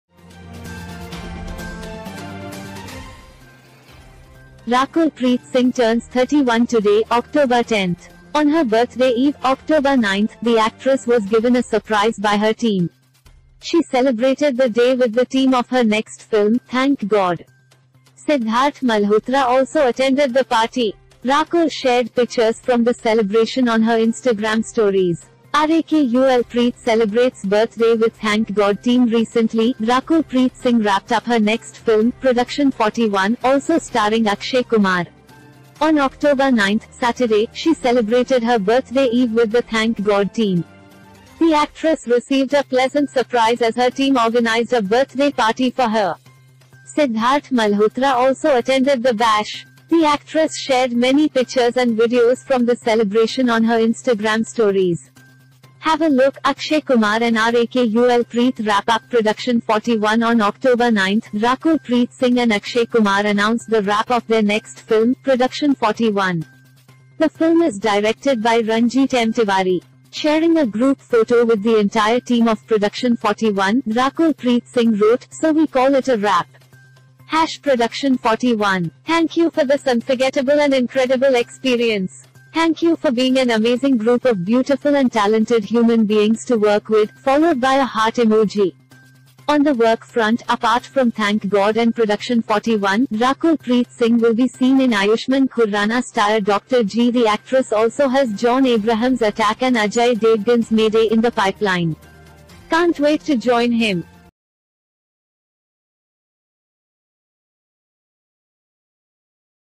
Rakul Preet celebrates birthday with Sidharth and Thank God team
0:10 Rakul Preet celebrates birthday with Sidharth and Thank God team